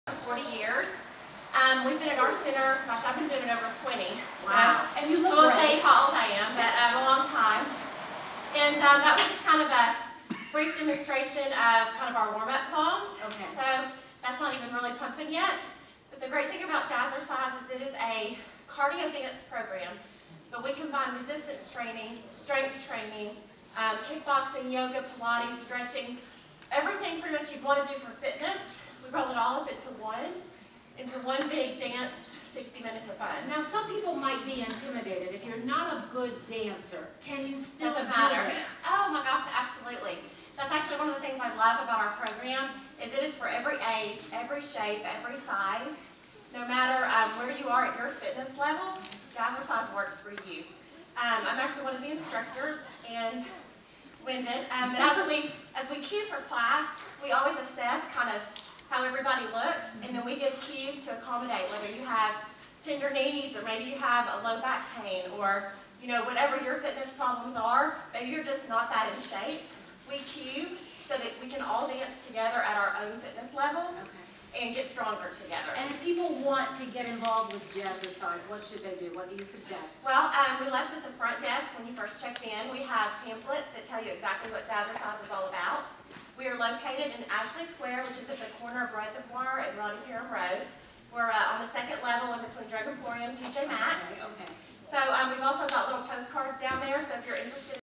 Jazzercise- Little Rock
Let's Dance 2 Health Day invites Arkansans’ to embrace the movement of dance as a fun and positive way to maintain good health and stay fit. us on Facebook Follow us on Twitter